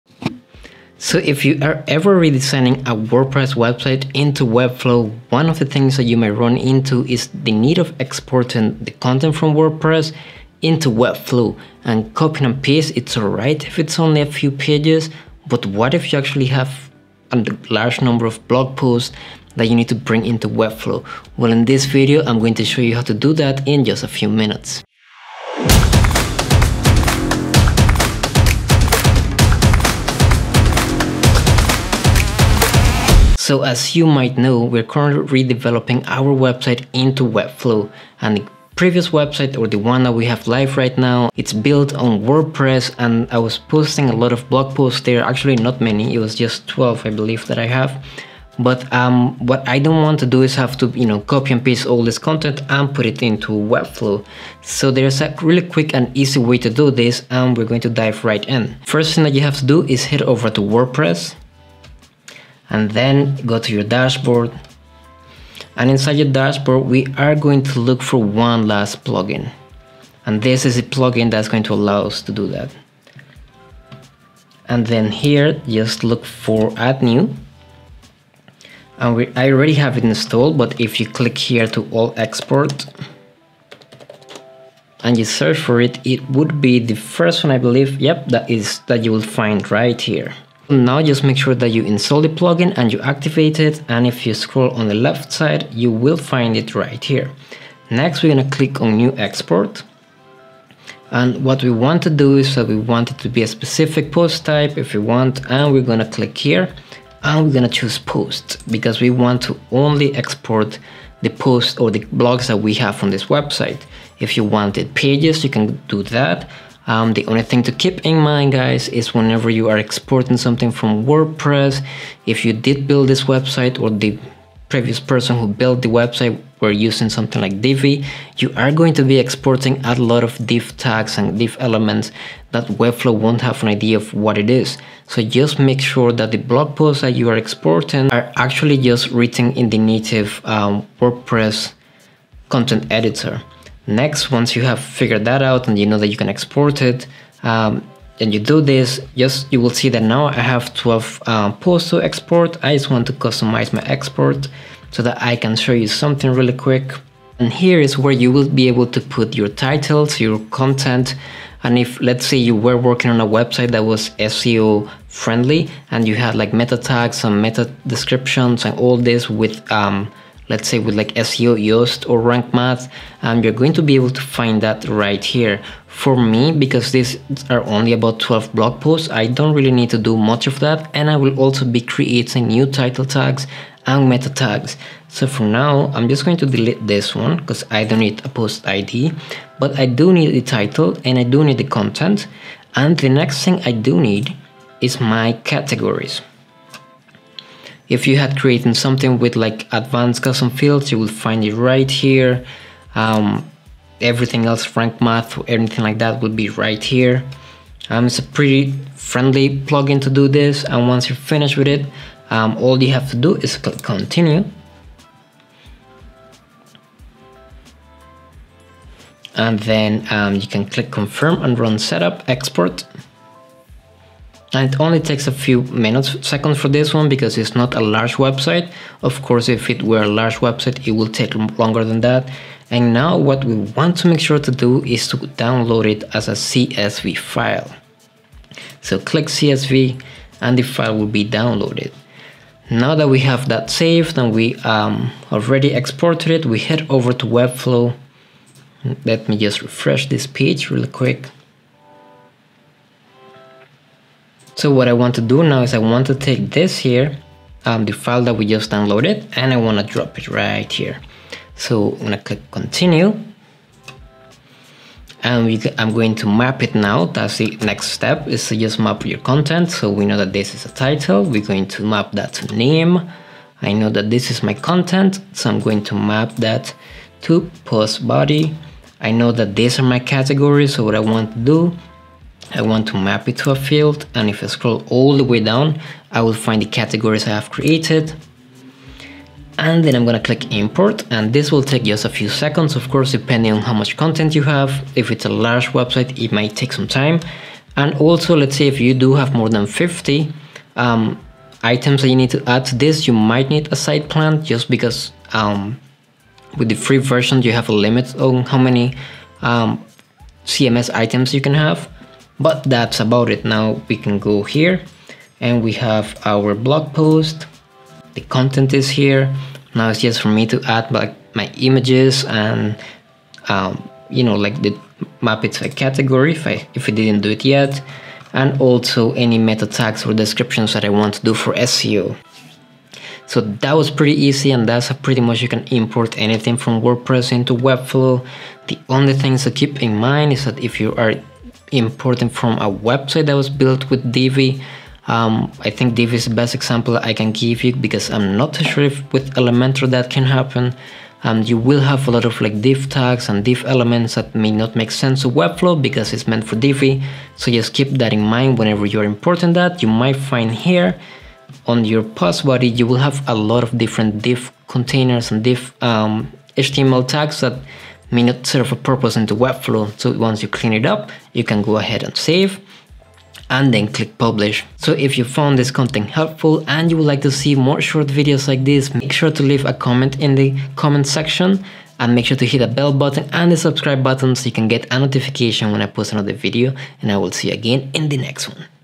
How to export from Wordpress to Webflow
If you are redeveloping a WordPress website into Webflow, you might run into having to export content from WordPress and import it in Webflow.

In this video I am showing you how you can do this using one single WordPress plugin, a CSV import file and Webflow's import option. This will only take a few minutes, but it all depends on how much content you are exporting from WordPress.

If you are exporting content from a website built using Divi into Webflow, make sure to clean up all the unnecessary HTML tags that might be added.

Chapters:
0:00 Video Topic Intro
00:40 Why you might need to do this
01:01 WordPress
01:10 Download Wordpress Export Plugin
01:17 Search for All Export
01:30 How to export from Wordpress
04:10 Mapping Fields inside Webflow
05:10 Tips and Thoughts

THE MUSIC IN ALL MY VIDEOS:
Epidemic Sounds | Great music for those doing Youtube Videos
Item ID:                         B3KYEPN
Author Username:                 LuckyBlackCat
Licensee:                        Leopoldo Pirela